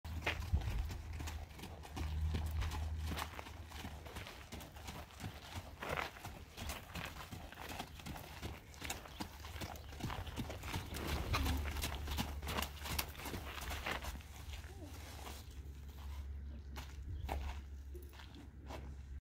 Pharao lunging at Canadian Hay Ranch
Registered Canadian Gelding for sale trained to ride for an experienced rider. He would be excellent for someone who wants to do arena work and ride western.